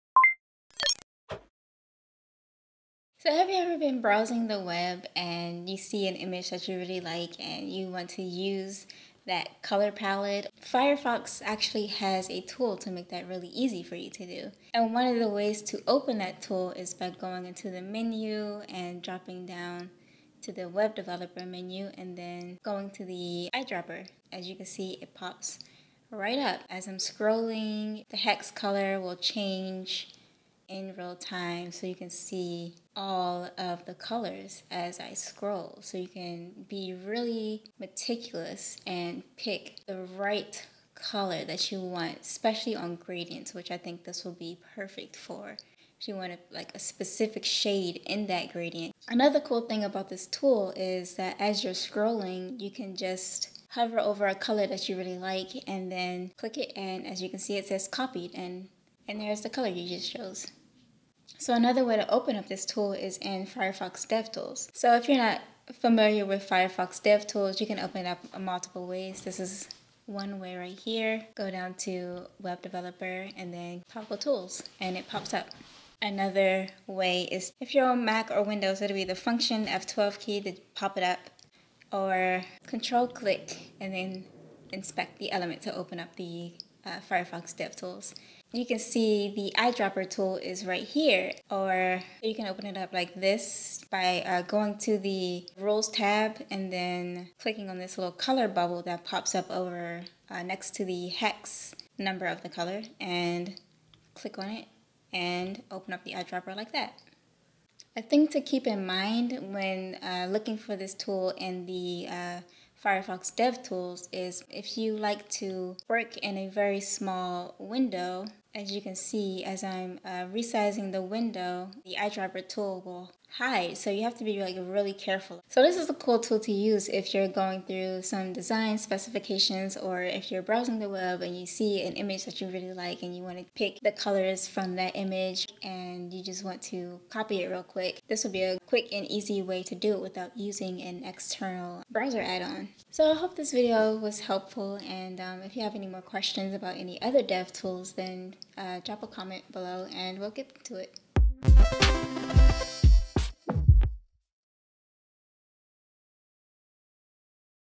Capture Any Color on the Web with the Firefox Eyedropper
Have you ever wanted to grab a color from a webpage or an image? Firefox has a tool for that!